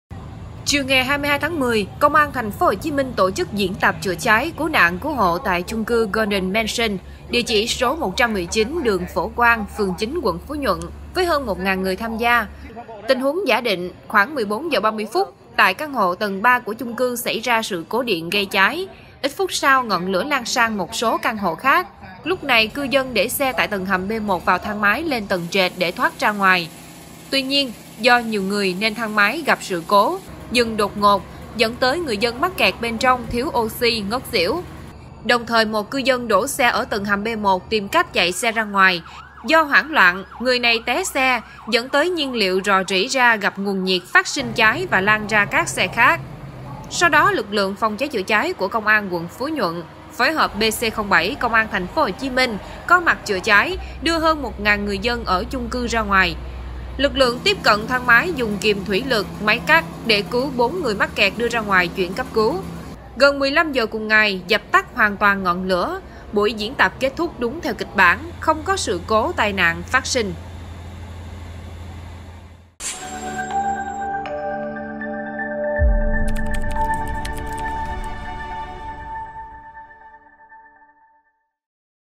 Hơn 1.000 người diễn tập chữa cháy ở chung cư Golden Mansion
Chiều 22-10, Công an TPHCM tổ chức diễn tập chữa cháy, cứu nạn, cứu hộ tại chung cư Golden Mansion với hơn 1.000 người tham gia.